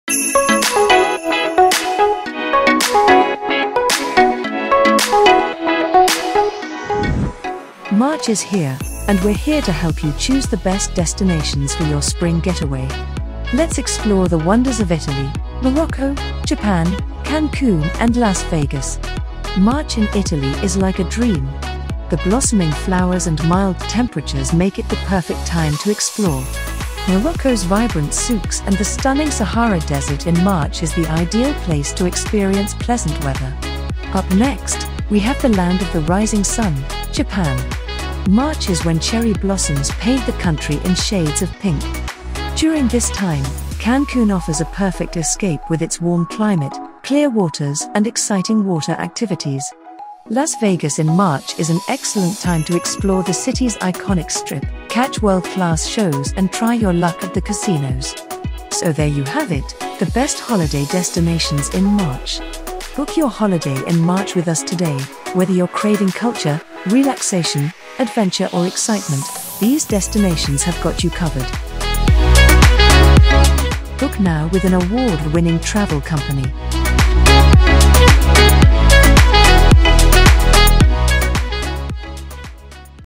Best Places To Visit In March | March Holiday Destinations
Wondering where to go on holiday in march? We're here to help you choose the best destinations for your March getaway. Let’s explore the wonders of Italy, Morocco, Japan, Cancun and Las Vegas!

ATOL & ABTA protected ✅